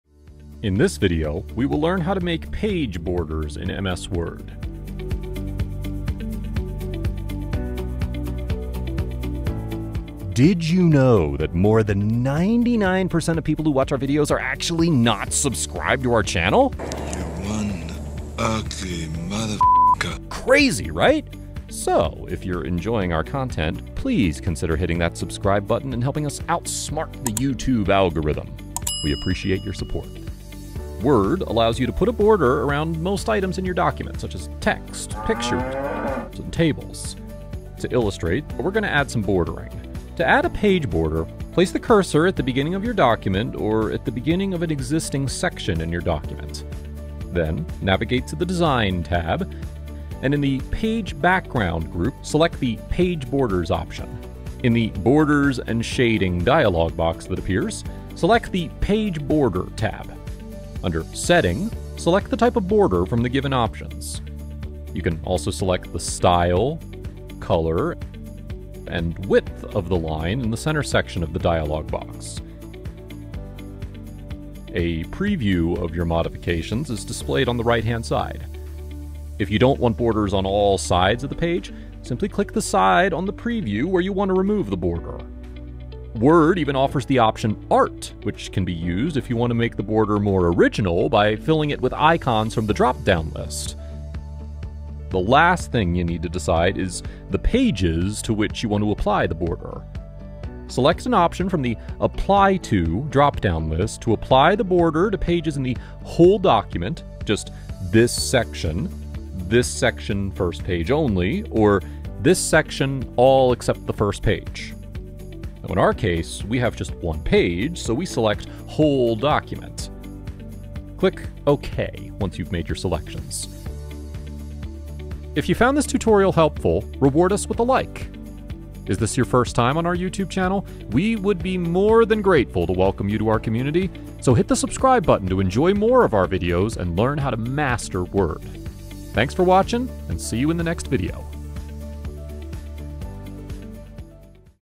How to add Border in Microsoft Word
In this video, we'll learn how to add Borders in Microsoft Word.
In Microsoft Word, borders are the set of lines, design shapes, images, text boxes, boundaries, and colorful graphics that are added in Word to make documents more visually appealing. Using a page border, you can customize the style and thickness of the page. Word allows you to put a border around most items in your document, such as text, pictures, and tables.